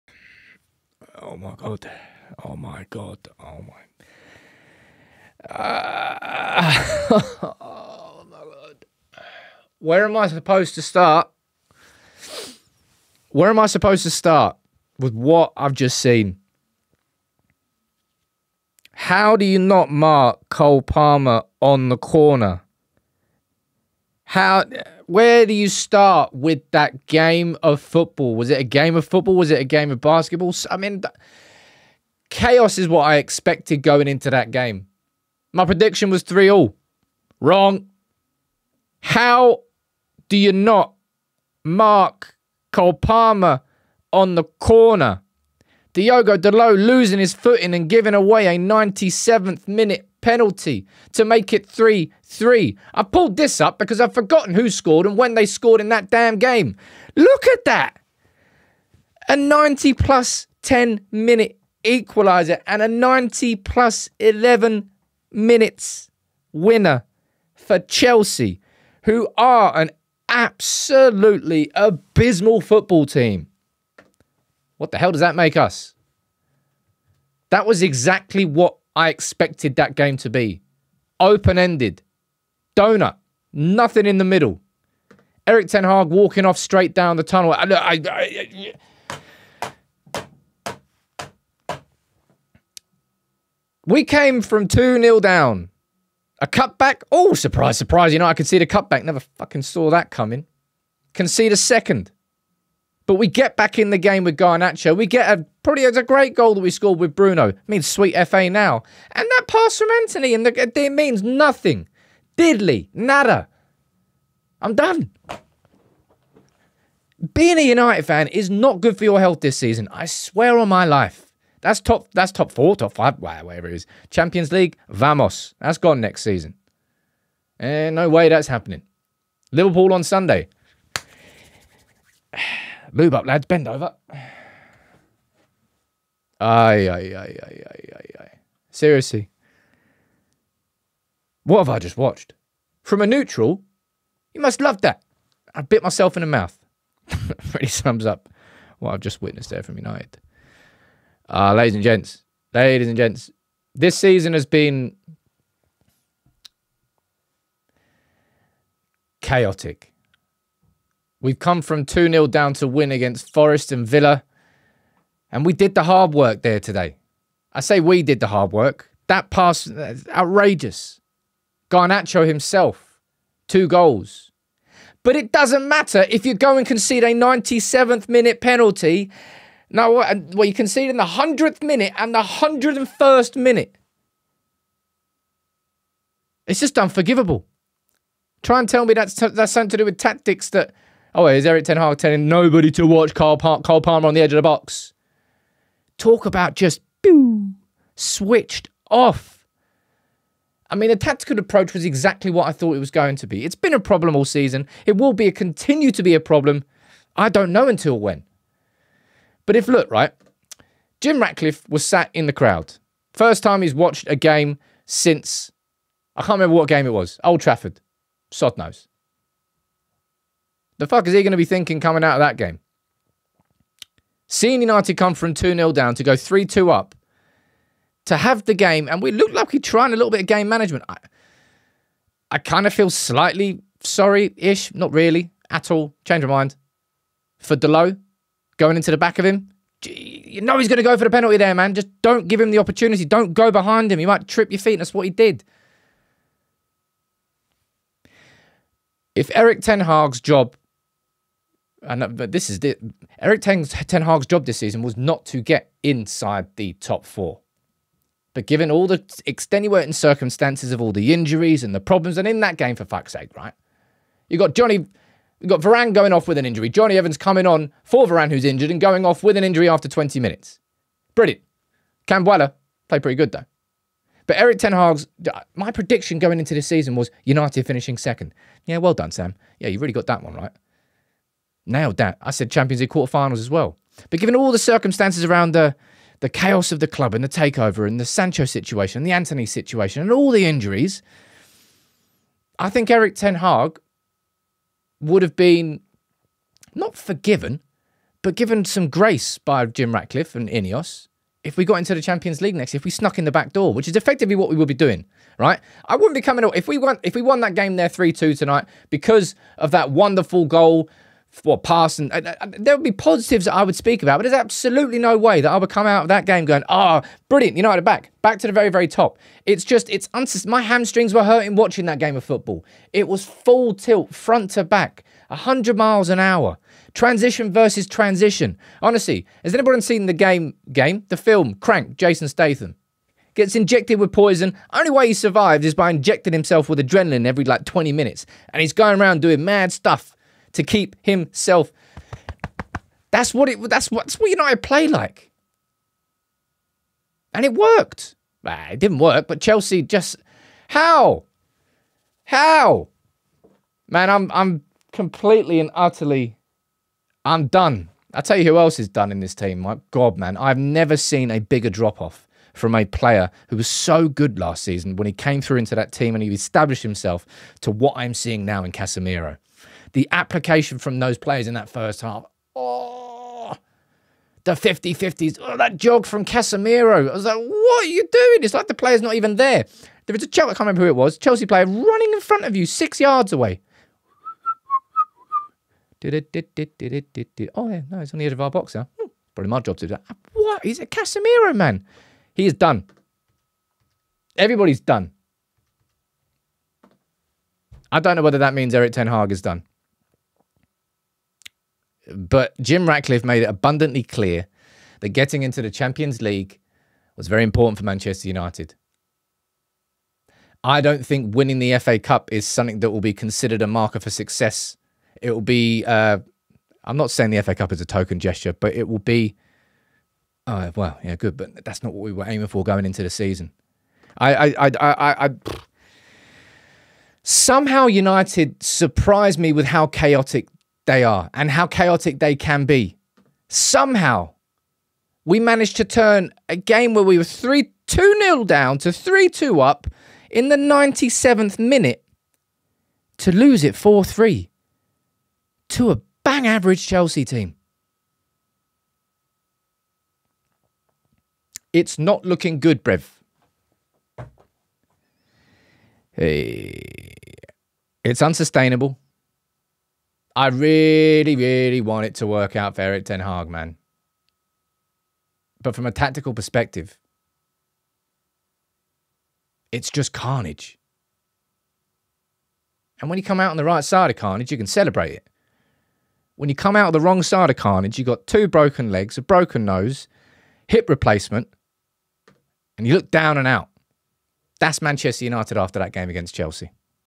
I AM COMPLETELY DONE | CHELSEA 4-3 MAN UTD | Unsustainable Ten Hag Chaos
I am done. How. How have Manchester United lost to Chelsea. I'm done. Speechless. Vamos. Gone. End the season, please. Where's the humanity?